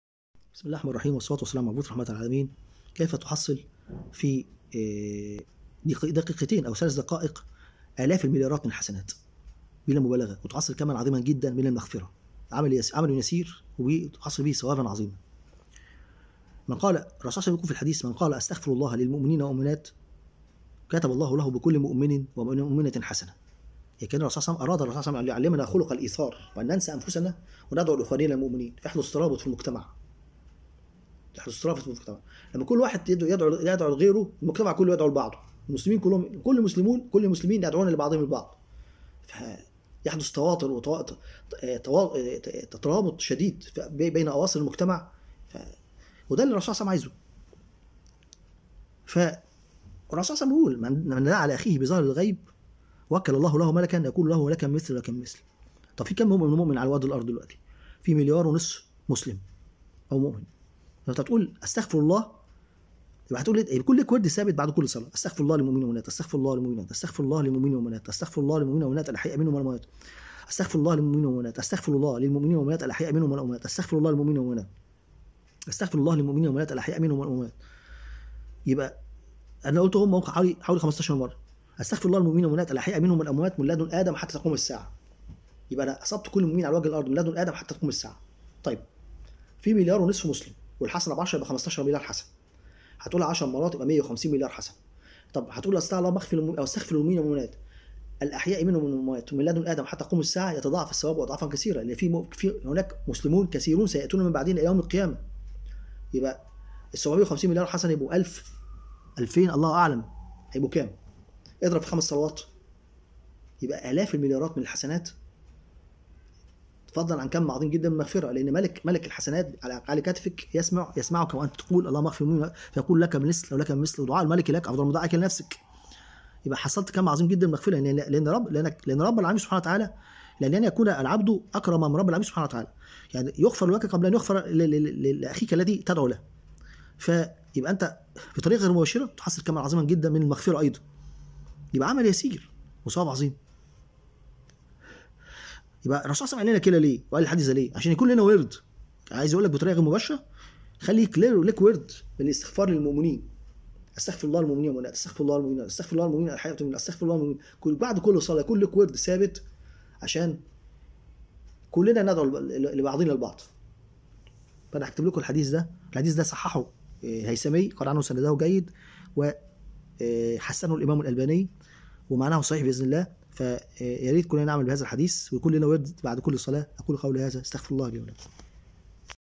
كيف تحصل ٣٠٠ مليار حسنة في ٣٠ ثانية و كماً عظيماً جدا من المغفرة قراءة وصف الفيديو هام جدا جدا
ttps://youtu.be/fLCohbr9Gro

قناة الشيخ حاتم مسعد

عن عبادة رضي الله عنه قال رسول الله صلى الله عليه وسلم : ( من استغفر للمؤمنين والمؤمنات كتب الله له بكل مؤمن ومؤمنة حسنة ) رواه الطبراني في "مسند الشاميين" (3/234)
قال الهيثثمي إسناده جيد و قال الألباني حديث حسن